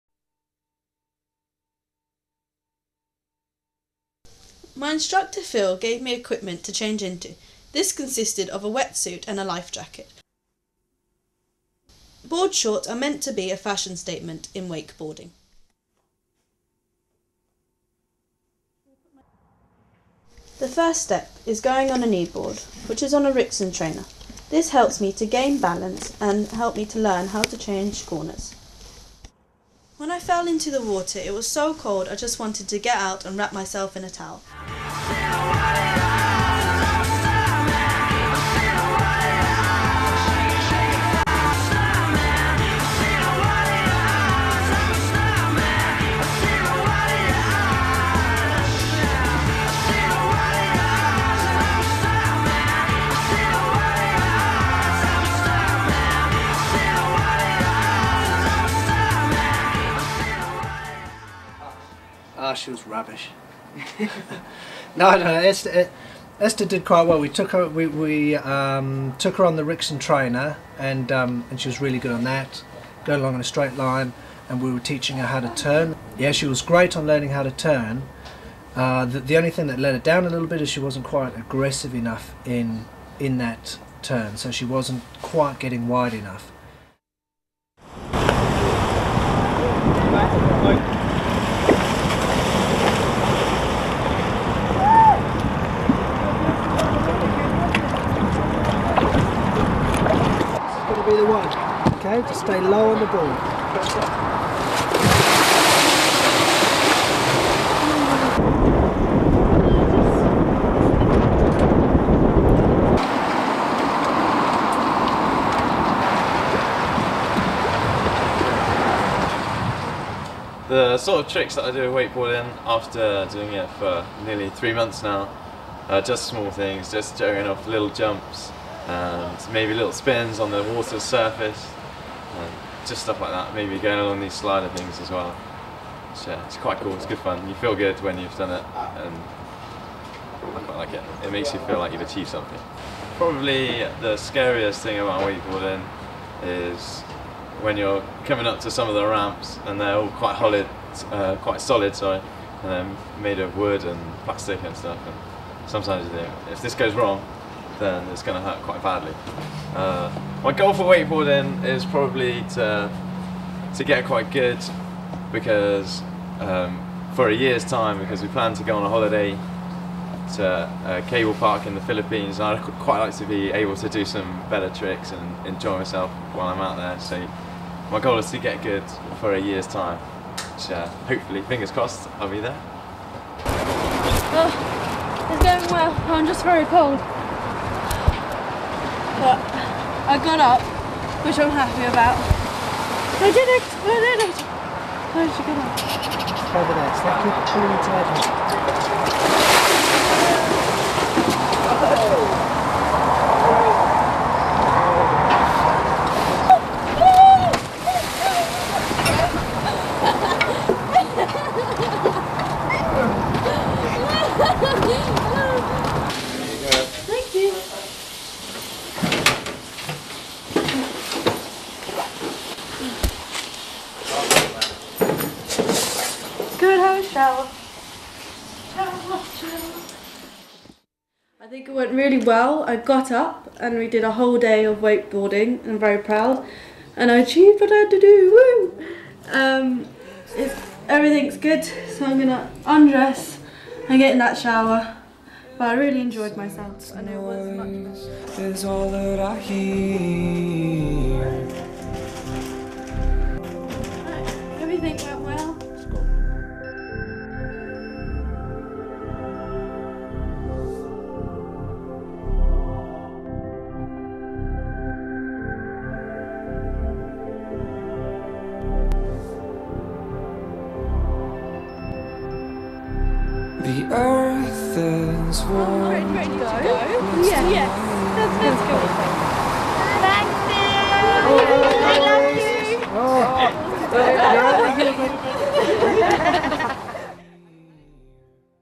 Wakeboarding Documentary Part 2/3
Documentary on Wakeboarding. This follows the the Story of Esther, who tries to stand up on a wakeboard within 2 weeks. Done as part of a Single Camera Documentary unit for university. Part 2/3